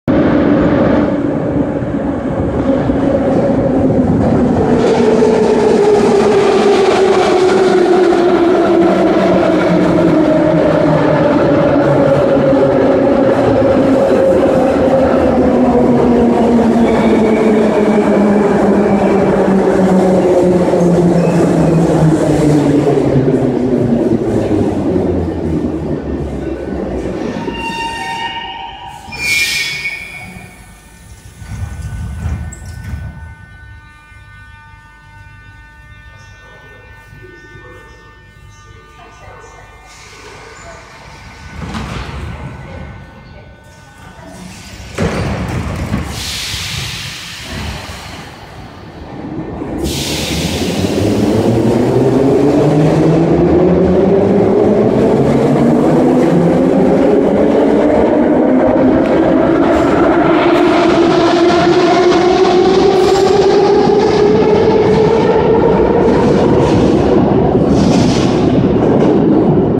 81-717/714.5М Перекраска Московский транспорт На станции Кожуховская
ОПИСАНИЕ

Поступил ТЧ-15 Печатники